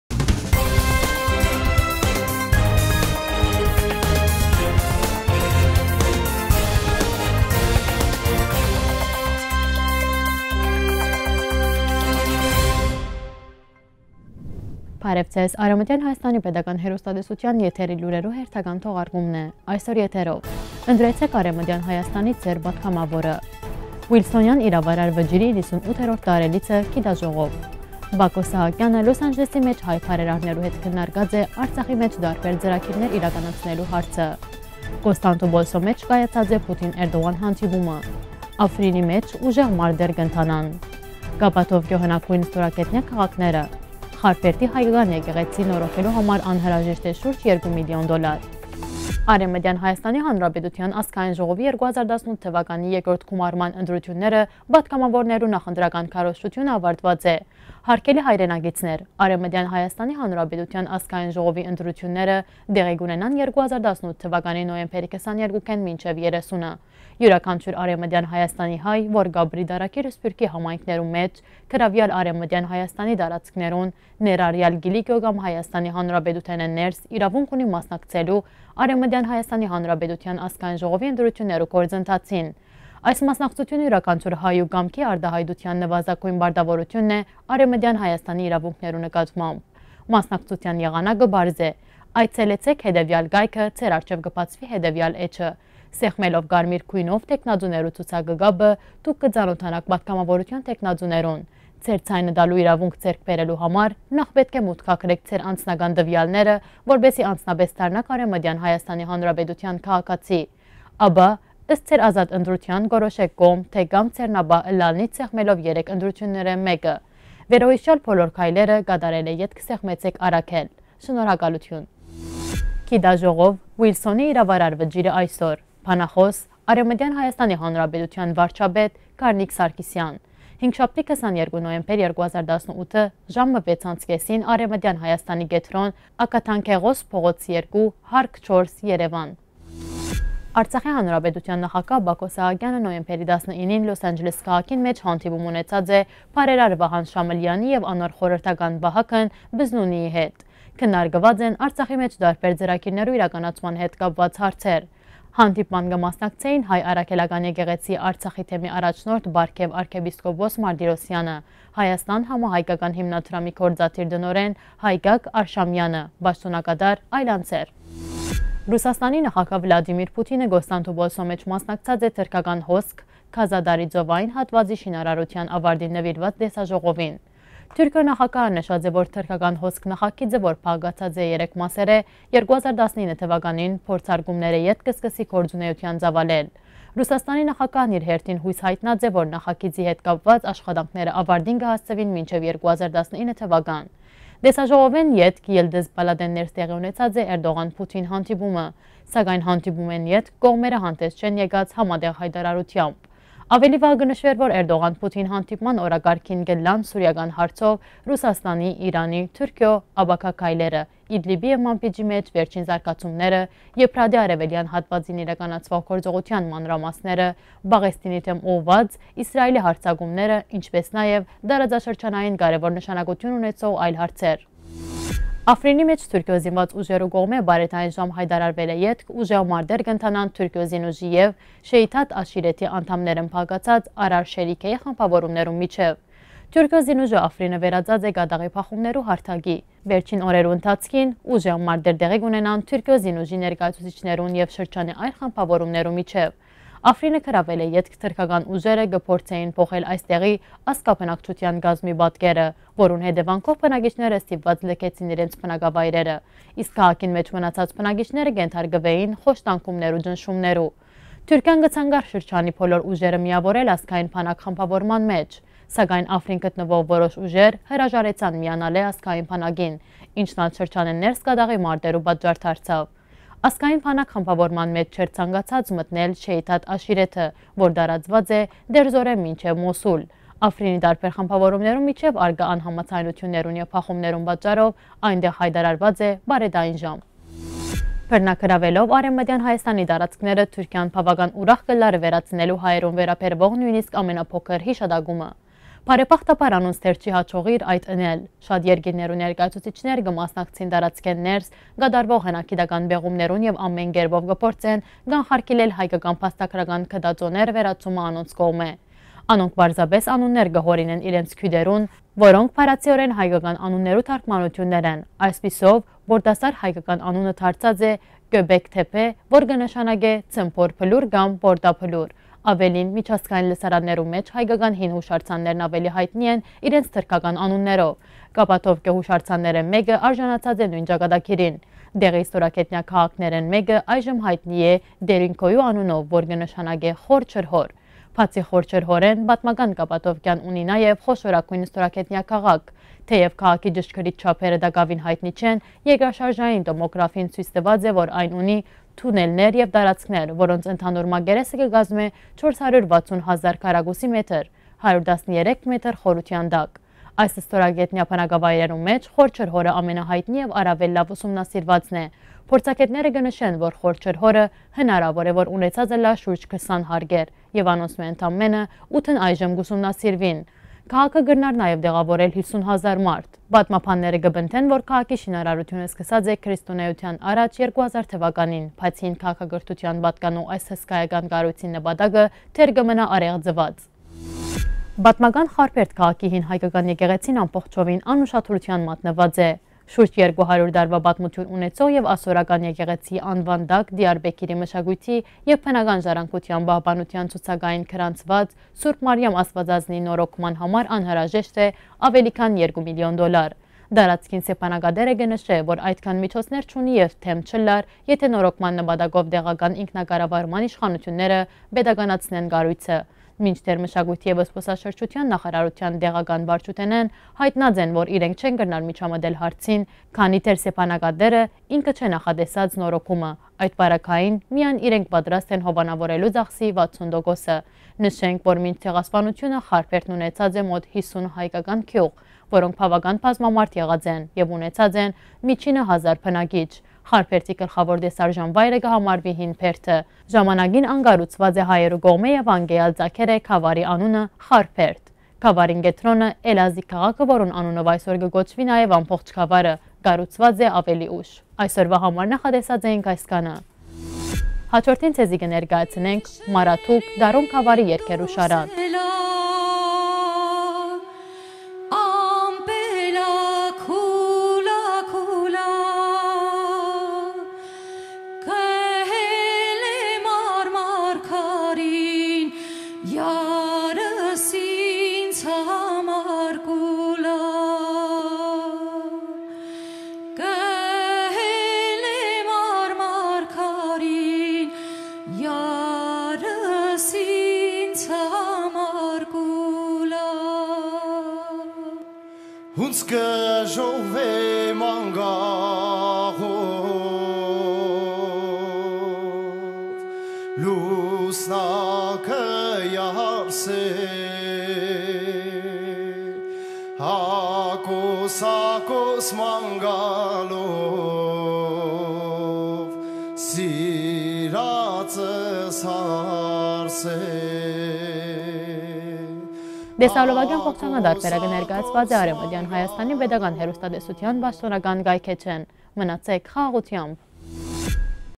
Լրահոս 2018-11-20
1.Ընտրեցէք Արեւմտեան Հայաստանի Ձեր պատգամաւորը
2.Ուիլսոնեան իրաւարար վճիռի 98-րդ տարելիցը․ գիտաժողով
3.Բակօ Սահակեանը Լոս Անջելըսի մէջ հայ բարերաներու հետ քննարկած է Արցախի մէջ տարբեր ծրագիրներ իրականացնելու հարցը
4. Կ․ Պոլսոյ մէջ կայացեր է Պուտին-Էրդողան հանդիպումը
5.Աֆրինի մէջ ուժեղ մարտեր կ՛ընթանան
6.Կապադովկիոյ Հնագոյն Ստորգետնեայ Քաղաքները
7.Խարբերդի հայկական եկեղեցին նորոգելու համար անհրաժեշտ է շուրջ 2 մլն դոլար